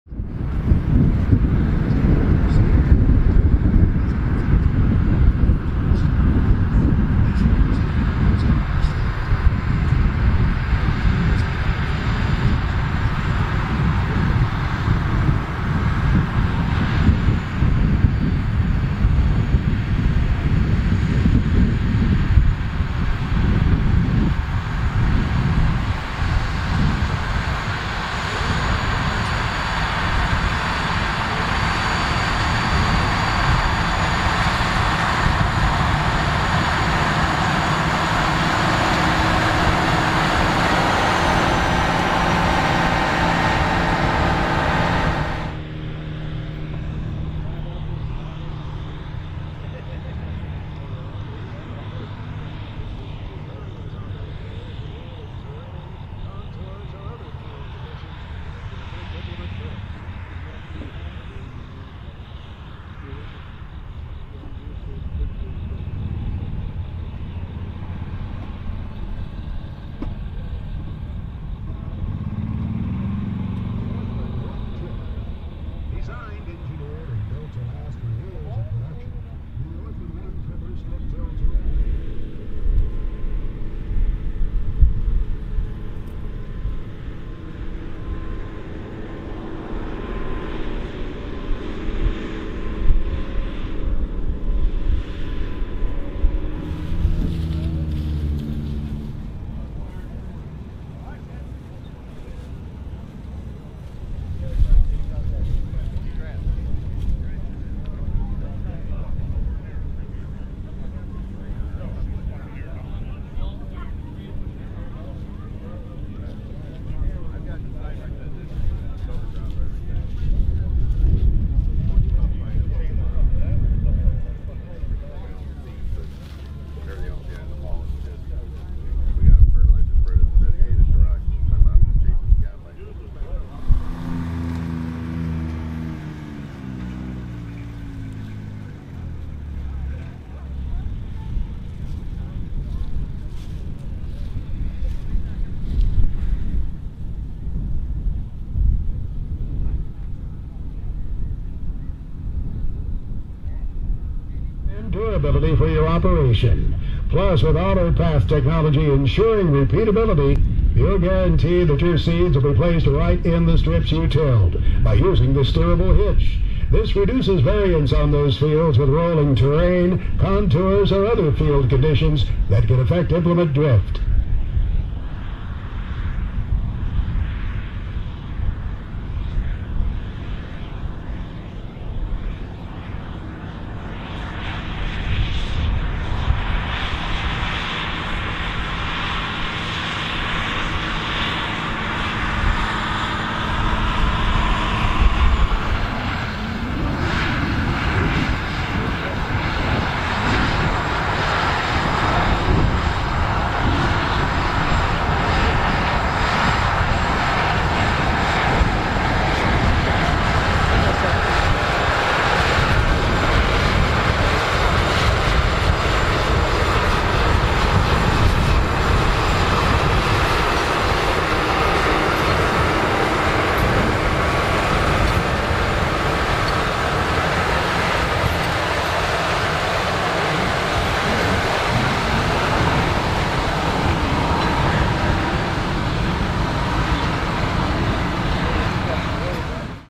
John Deere 9RX 590 with John Deere ST16
In this video you will see a John Deere 9RX 590 with a ST16 Strip-Till Solution.